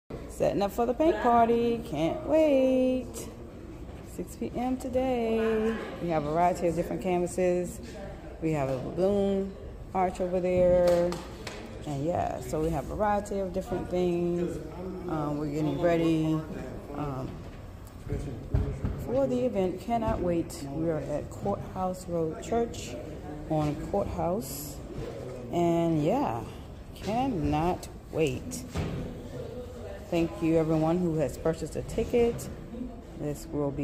Praise and Paint Party - Courthouse Rd Church
We create a fun worship and praise atmosphere for all attendees. While enjoying the therapeutic benefit of painting.